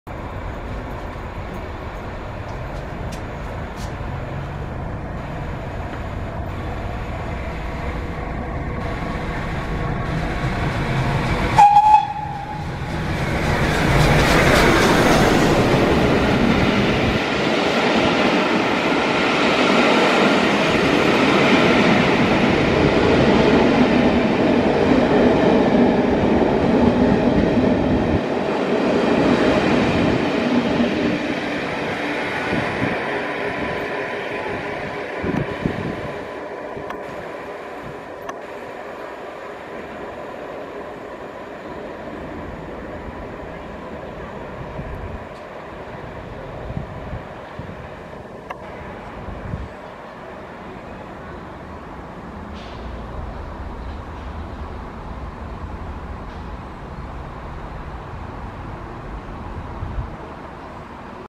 7029 Clun Castle - The Shakespeare Express passes Olton Station
The August Bank Holiday Sunday 25th August 2024.

I saw some times for when The Shakespeare Express would pass stations in Solihull, so got the no 4 bus up from Solihull Town Centre, with time to spare to see The Shakespeare Express pass Olton Station with 7029 Clun Castle.

As with 5043 Earl of Mount Edgcumbe the month before, I later saw the steam locomotive at Birmingham Moor Street platform 1, waiting to go back through the Snow Hill Tunnel to Birmingham Snow Hill Station (carriages usually waiting at platform 2).